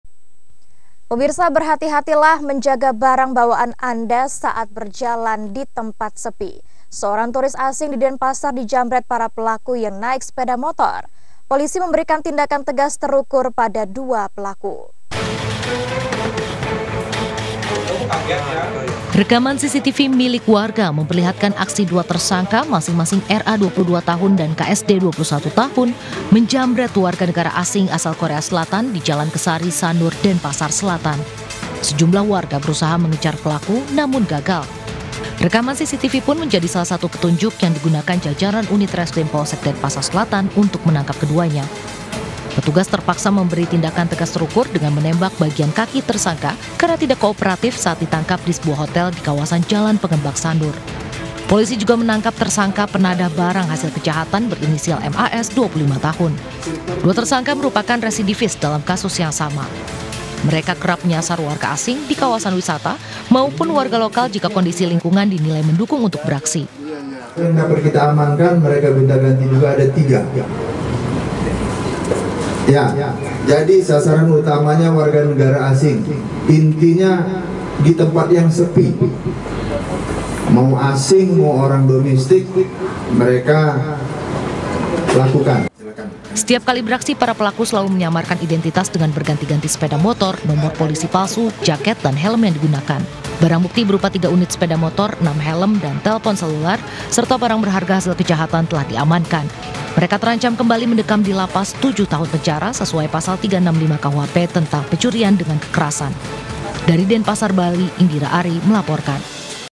Waspada! Turis Asing Di Denpasar Dijambret Pelaku Yang Naik Sepeda Motor
Denpasar - iNews Bali, 24 Oktober 2024, Waspada! Turis Asing Di Denpasar Dijambret Pelaku Yang Naik Sepeda Motor